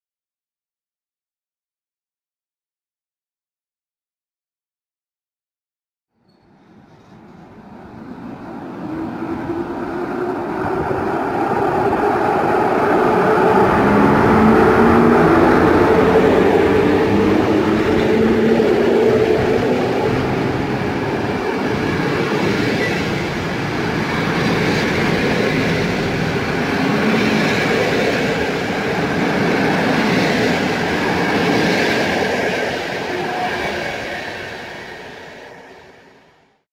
EP07-338 Zielony Trapez z IC 84102 Szyndzielnia do Bielska-Białej.
Gdyński Zielony Trapez EP07-338 z pociągiem IC 84102 Szyndzielnia relacji Ustka - Bielsko-Biała Główna odjeżdża z toru 2 przy peronie 2 stacji Żmigród w kierunku Wrocławia.

Województwo: Dolnośląskie
Miejscowość: Żmigród
Data: 27 Maj 2022r.
Godzina: 16:05
Przewoźnik: PKP Intercity
D29: 271 (Wrocław - Poznań)
Montaż: Camtasia Studio